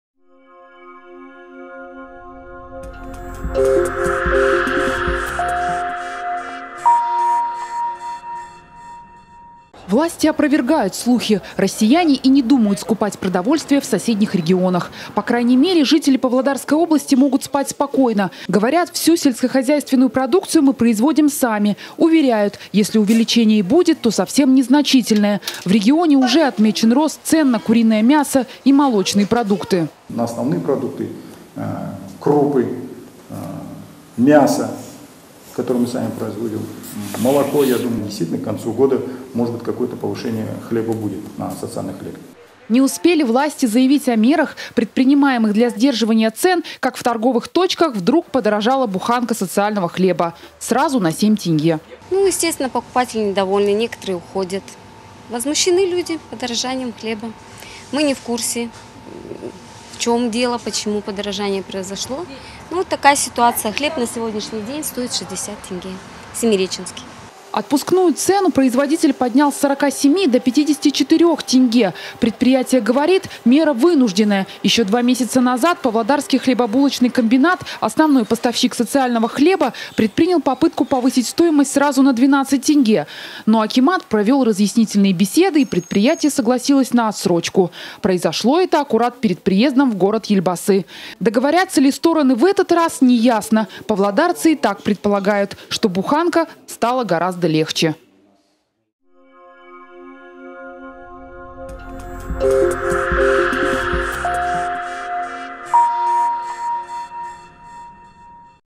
Наш ответ санкциям: в Павлодаре подорожал хлеб / 1612
В приграничных с Россией областях Казахстана ажиотаж. Люди уверены, в связи с санкциями ЕС и России, стоимость продовольственных продуктов резко повысится. Однако в Павлодаре руководство области заверило, цены останутся прежними, а в качестве доказательства в магазинах тут же подорожал хлеб.